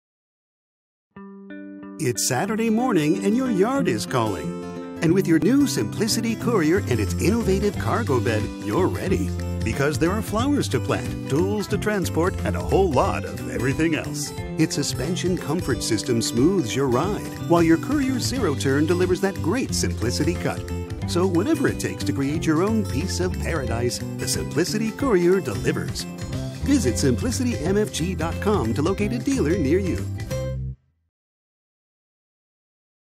Simplicity Courier™ Spring TV Commercial
With the Simplicity Courier™ Zero Turn Mower and its unique innovative cargo bed*, you’ll be ready to tackle all of the weekend chores waiting in your yard. The Suspension Comfort System™ offers a smooth ride while you get that great Simplicity cut. See it in action in this spring TV commercial.